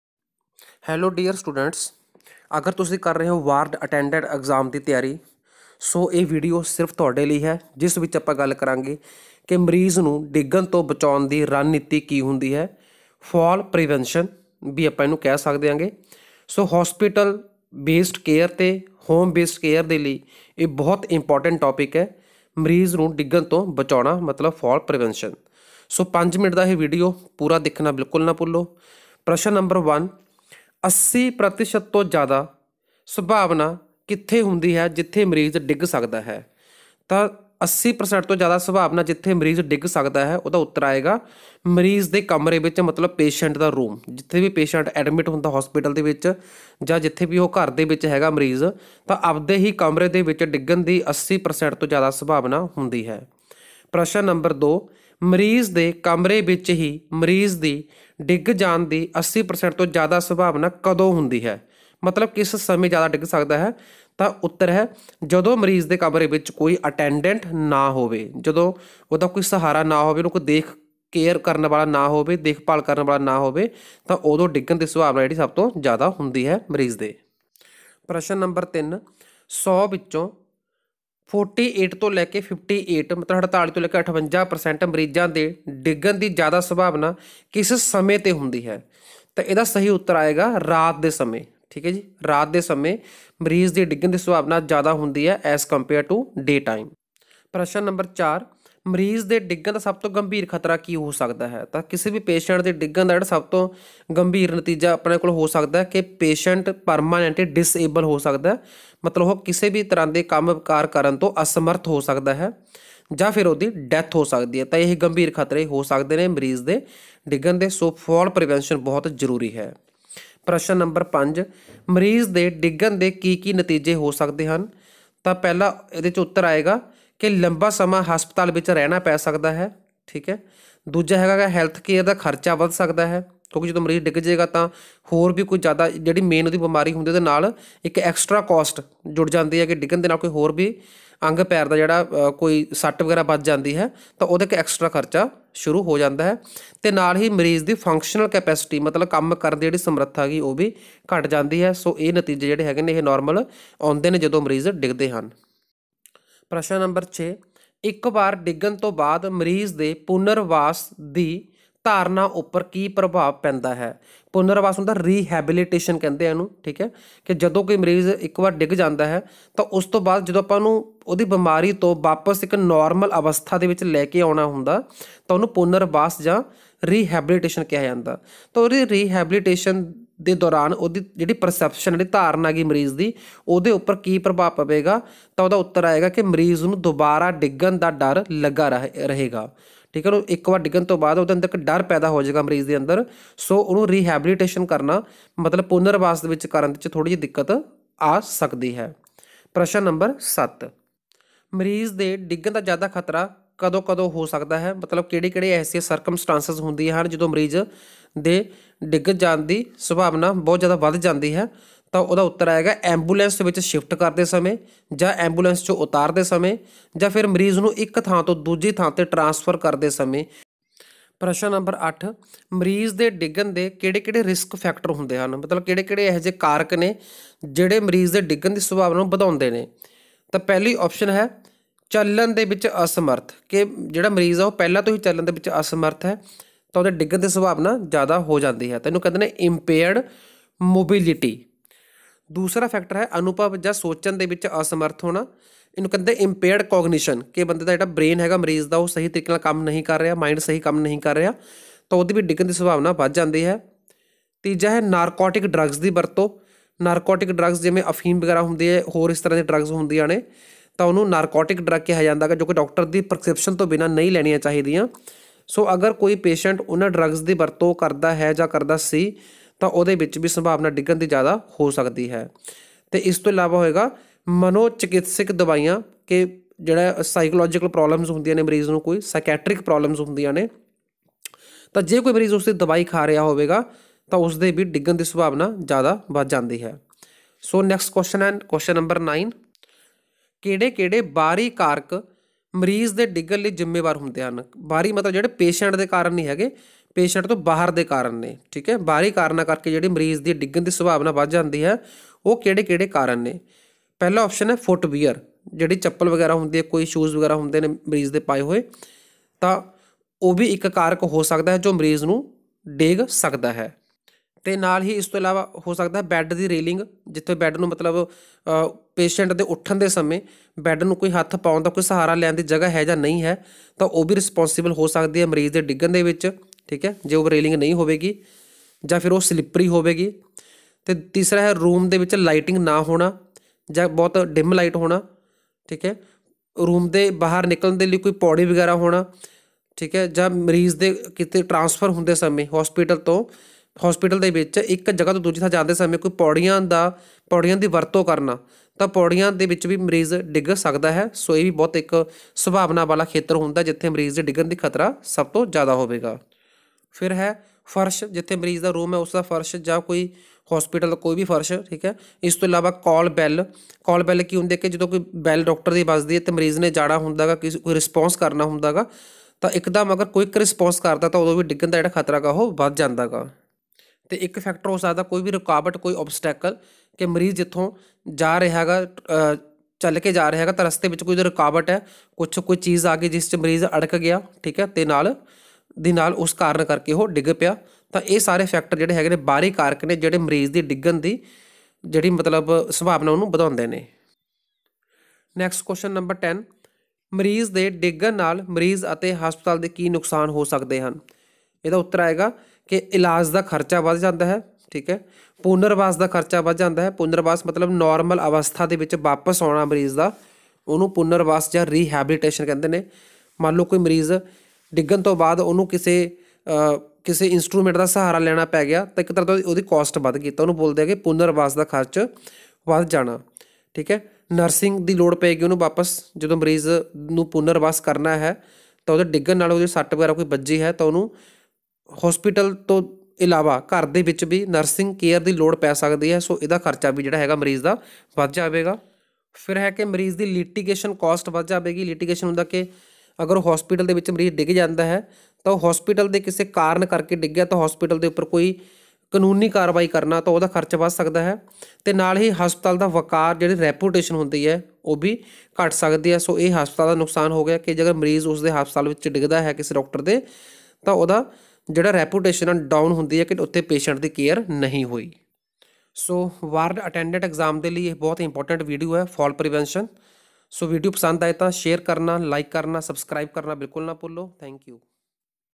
Home based care# patient fall prevention#hospital based care patient safety#fallprevention strategy
This video explains all about patient fall prevention in home based and hospital based care. Video is important for ward attendant exam 2020 and nursing students.

ward attendant exam :